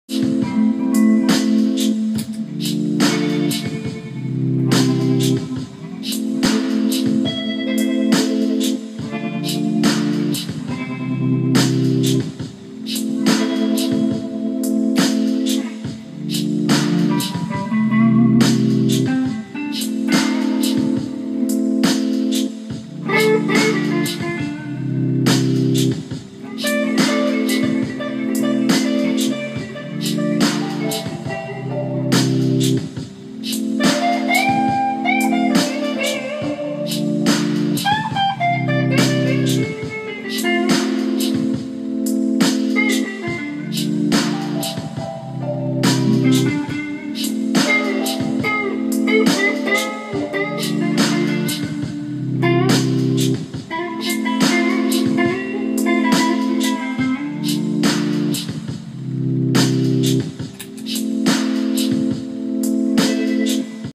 s w e l l s
improv blues jam over a track i wrote last night